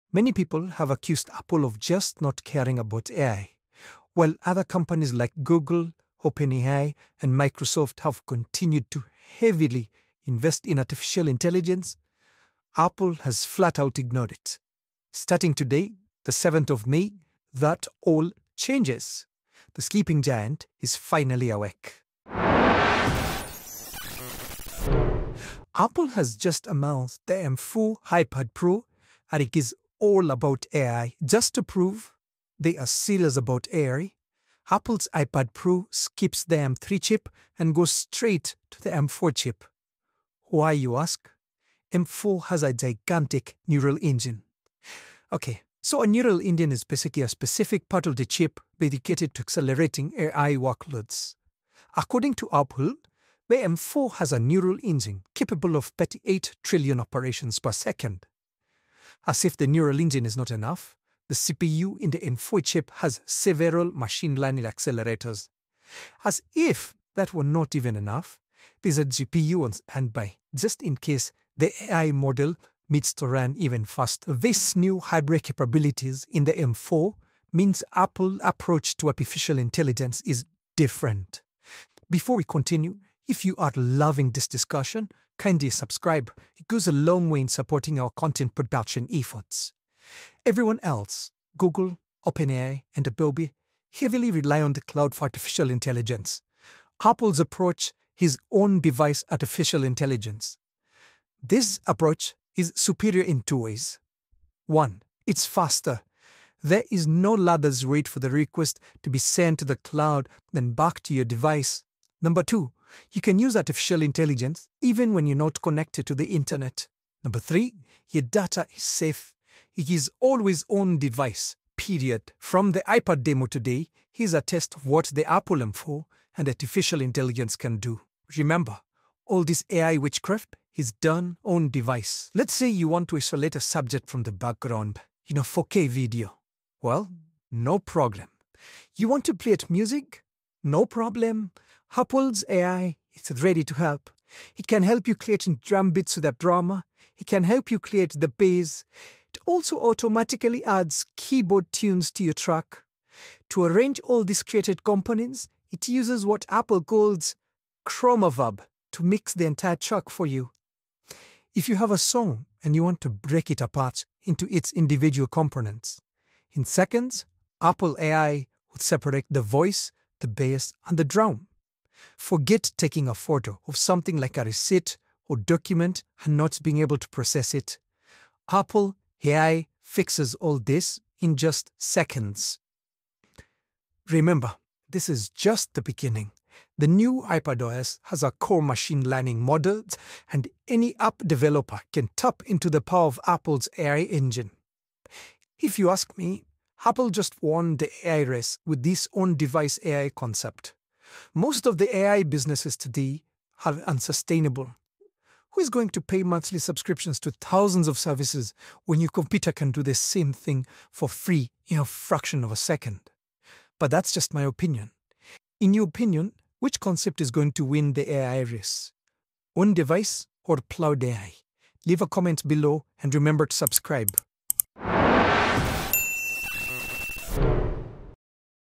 Apple wins the AI race: The new M4 iPad pro is all they needed.
In the just concluded Apple event, Apple announced the iPad pro. Subtle within the announcement was apples, new A.I strategy. This new strategy catches Google, OpenAI, and Microsoft face down.

0:00 - Introduction
0:34 - The M4 chip.
2:03 - Apple's A.I on show
3:15 - Conclusion